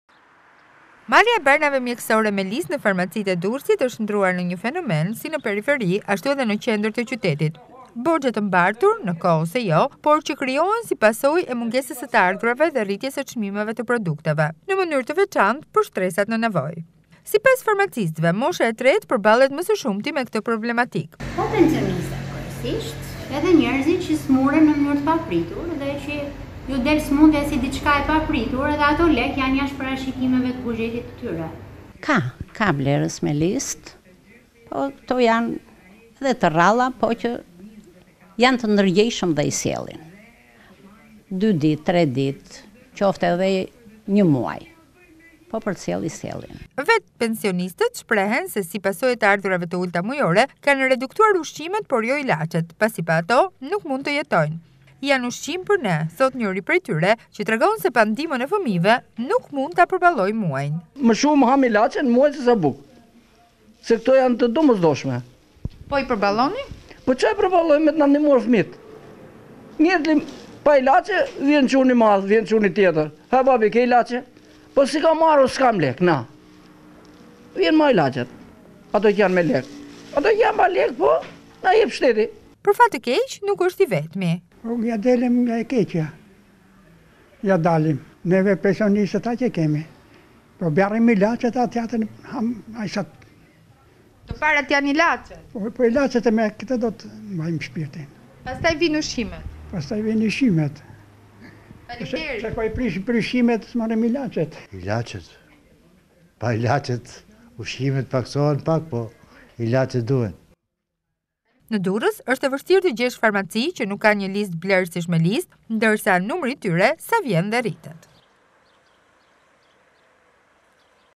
Ilaçet me listë, fenomen në farmacitë e qytetit, pensionistët: Të parat ilaçet, pastaj ushqimet
Blerja e barnave mjekësore me listë, fenomen në farmacitë e qytetit, pensionistët: Të parat ilaçet, pastaj ushqimet